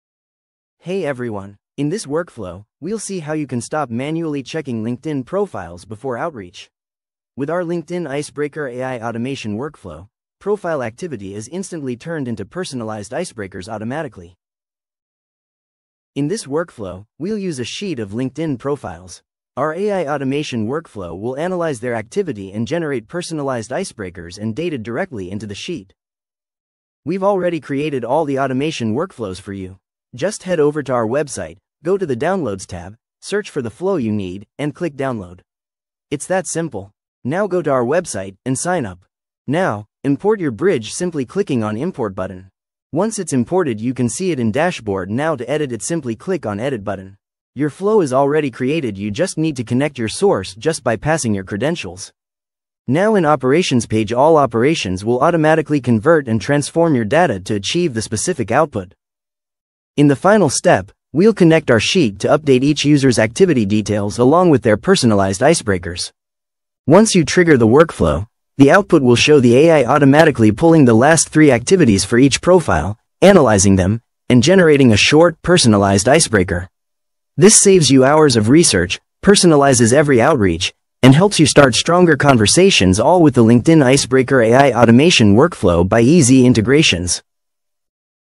How to Automate LinkedIn Lead Research & Generate Personalized Icebreakers?
Tired of spending hours researching LinkedIn profiles just to craft the perfect opening line? With eZIntegration’s AI-automation workflow, you can now create personalized icebreakers in just one click.

Here’s how it works:
eZIntegration analyzes the LinkedIn profiles you provide.

It tracks recent activities like posts, comments, and likes.
Based on these insights, it generates personalized icebreakers tailored for each lead.

This means your business can:
✅Save time on manual research
✅Build stronger first connections
✅Increase response rates with impactful, authentic icebreakers

With AI automation, LinkedIn outreach becomes smarter, faster, and more personal.

Ready to connect better and close more deals?